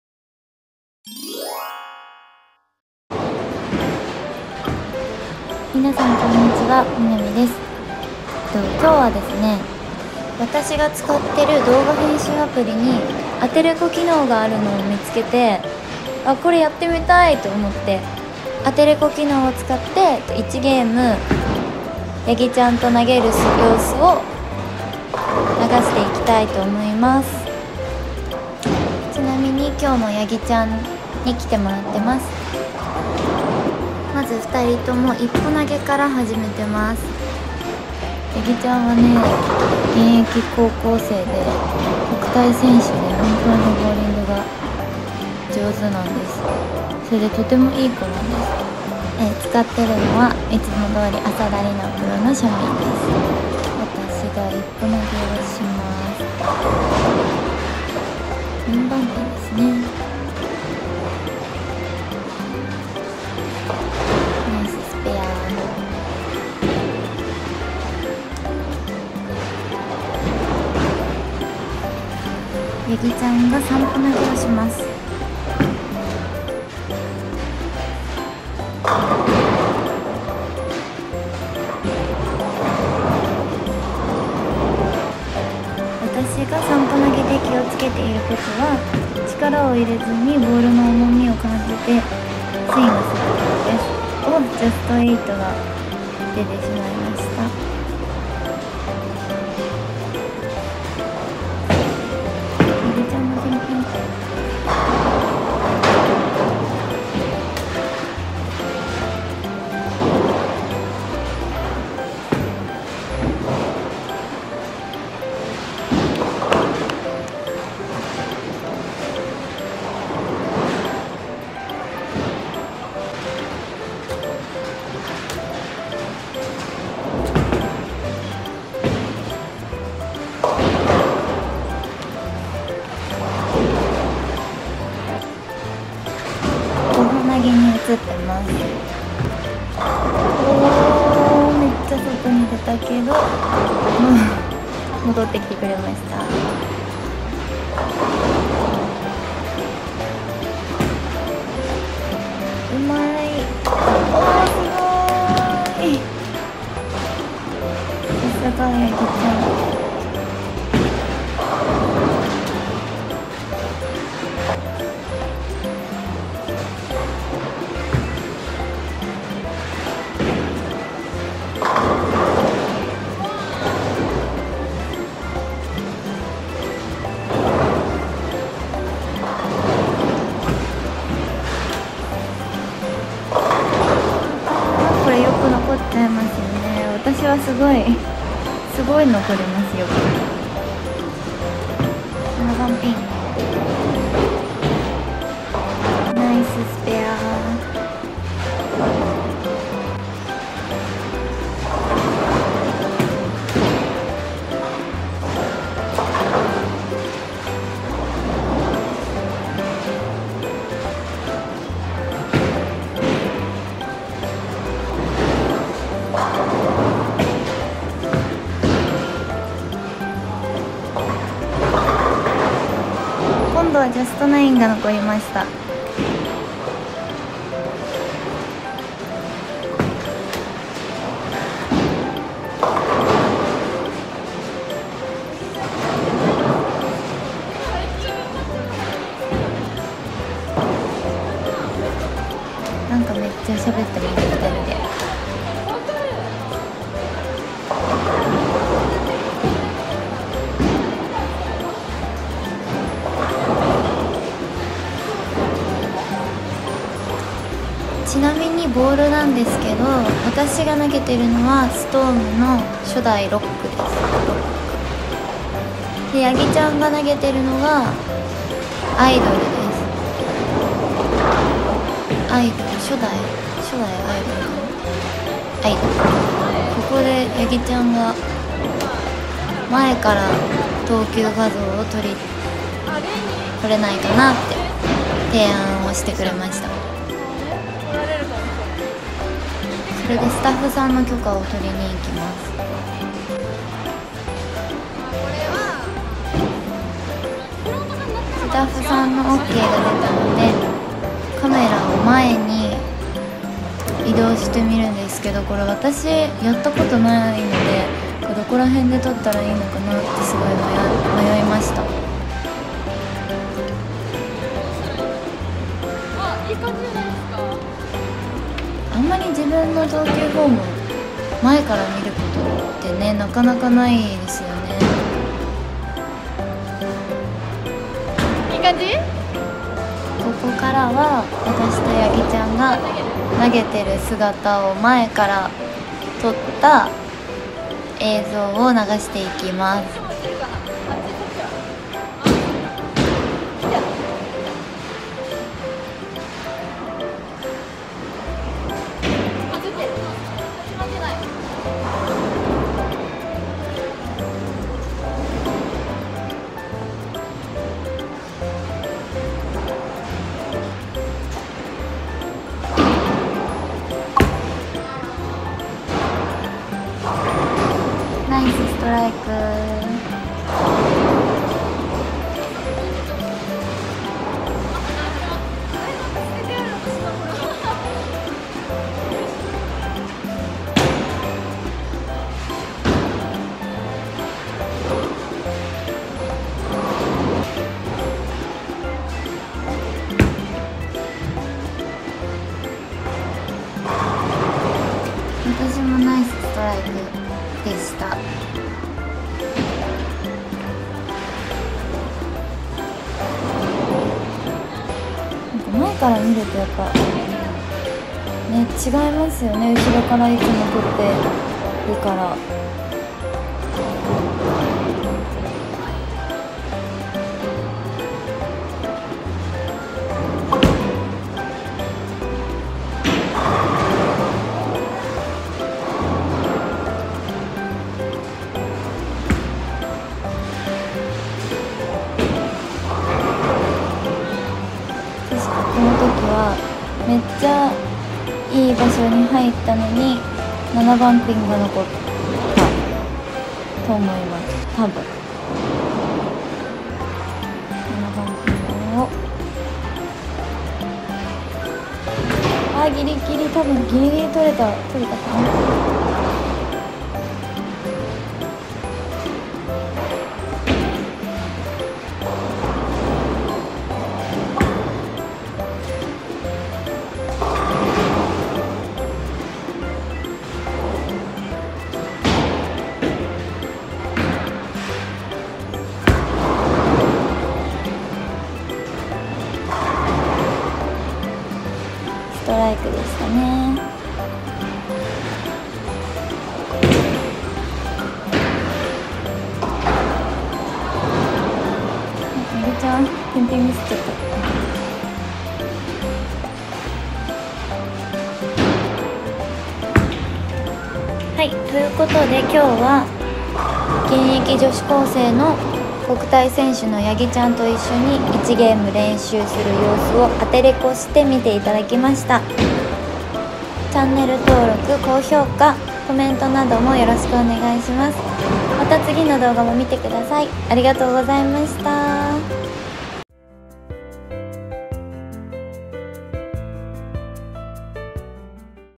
【JKとボウリング】編集ほぼ無しただの練習風景にアテレコ
国体選手の八木ちゃんと練習しているだけの動画に、
アテレコ機能使って音声つけてみました。